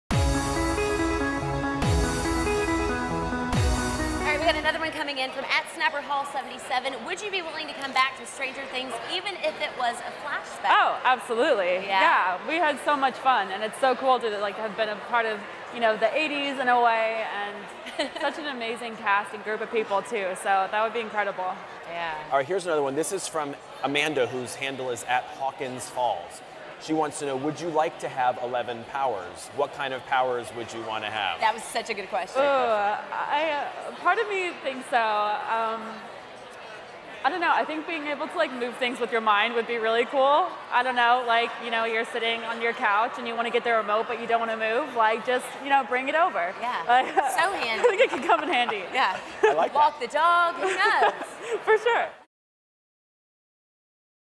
Shannon Purser Takes Fan Questions About "Stranger Things" | IMDb EXCLUSIVE
Shannon Purser answers some burning questions from IMDb fans about her character Barb on "Stranger Things."